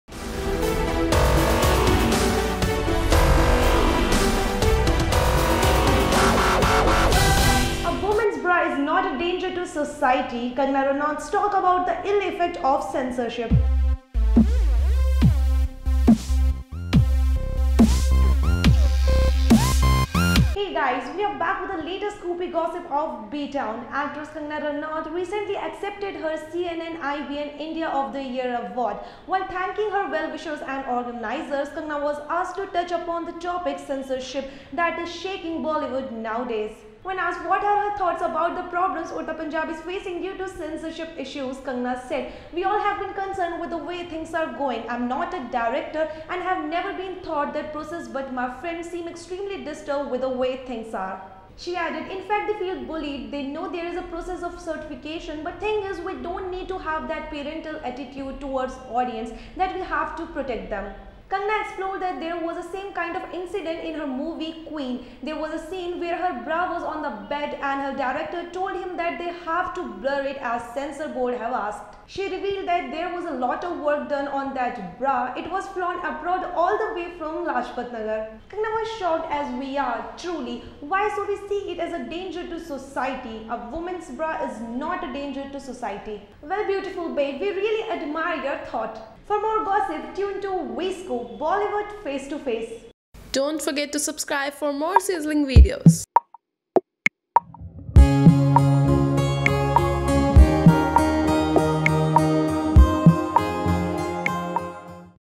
A woman Bra Is Not Danger | Kangana Ranaut
A woman’s bra is not a danger to society: kangana ranaut talks about the ill-effects of censorship